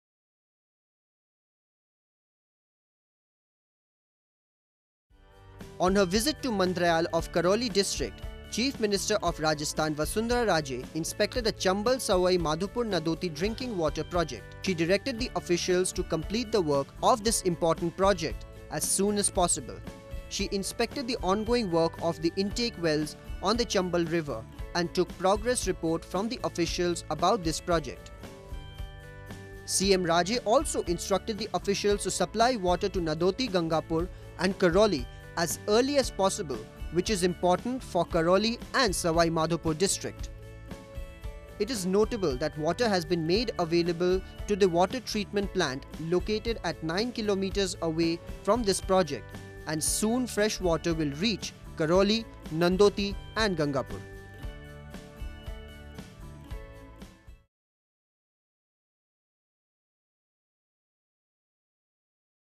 CM Raje inspects drinking water project at Mandryal
Karauli (Rajasthan) June 12 (ANI):- On her Visit to Mandryal of Karauli District, Chief  Minister of Rajasthan Vasundhara Raje  inspected the Chambal-Sawai Madhopur-Nadoti drinking water project. She directed the officials to complete the work of this important project as soon as possible. She inspected the ongoing work of the intake wells on Chambal River and took progress report from officials about this project. CM Raje also instructed the officials to supply water to Nadoti-Gangapur and Karauli as early as possible which is very important for Karauli and Sawai Madhopur district.It is notable that water has been made available to the water treatment plant located at 9 Km away from this project and soon fresh water will reach Karauli, Nadoti and Gangapur.